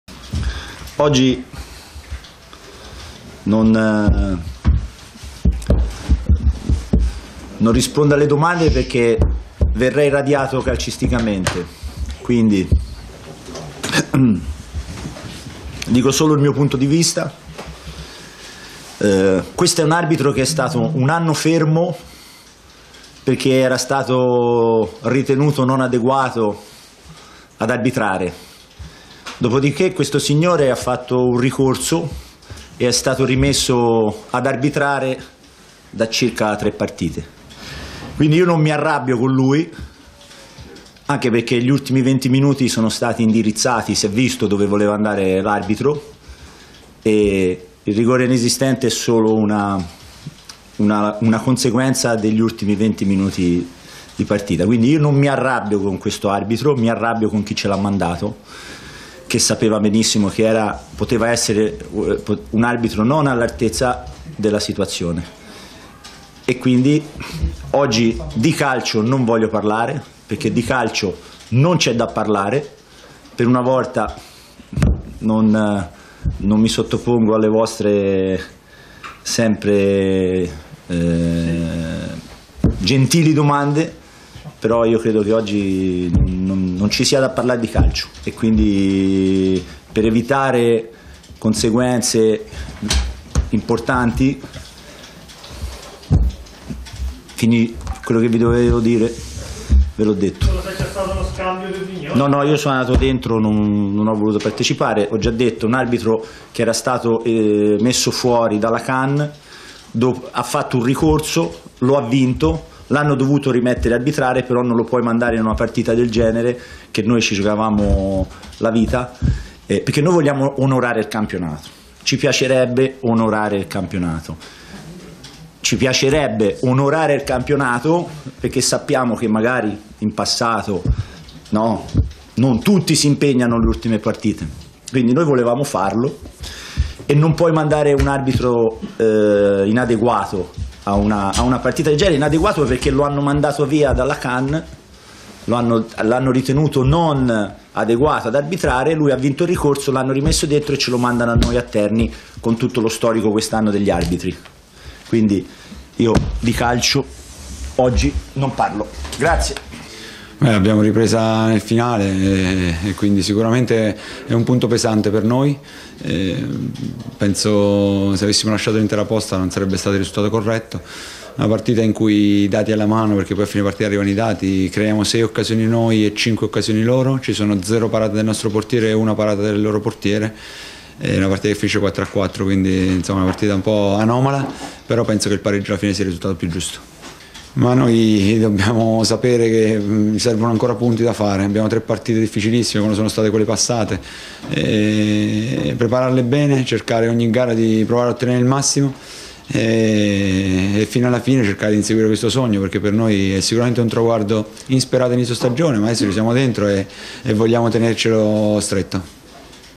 Ternana-Frosinone 4-4, Lucarelli e Grosso post gara
Serie B 2021-22